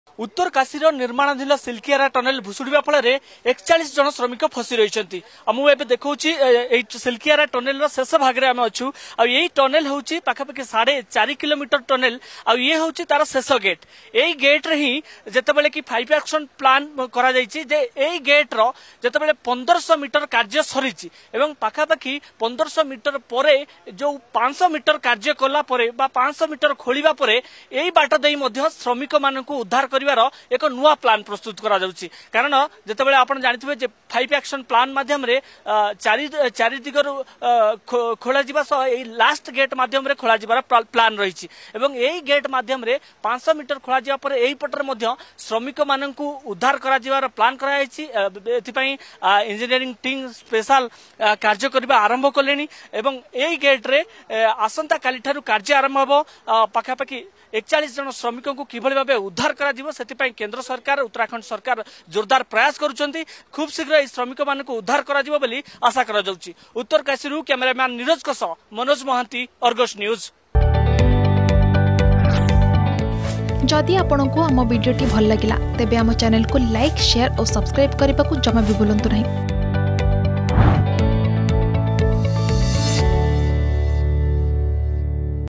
ଉତ୍ତରକାଶୀ ଟନେଲ ଭୁଶୁଡିବା ଫଳରେ ଫସିଛନ୍ତି ୪୧ ଜଣ ଶ୍ରମିକ || Uttarkashi Tunnel Collapse
ଉତ୍ତରକାଶୀ ଟନେଲ ଭୁଶୁଡିବା ଫଳରେ ୪୧ ଜଣ ଶ୍ରମିକ ଫସିଛନ୍ତି । ଶ୍ରମିକ ମାନଙ୍କ ଉଦ୍ଧାର ପାଇଁ କେନ୍ଦ୍ର ଓ ରାଜ୍ୟ ସରକାର ଜୋରଦାର ପ୍ରୟାସ କରୁଛନ୍ତି । ପ୍ଲାନ ବି ଅନୁସାରେ ଟନେଲର ଶେଷ ଭାଗରେ ୬୦୦ରୁ ୭୦୦ ମିଟର ଖୋଳା ଯାଇ ଶ୍ରମିକ ମାନଙ୍କ ପାଖରେ ପହଞ୍ଚି ପାରିବେ ଉଦ୍ଧାର କାରି ଦଳ ।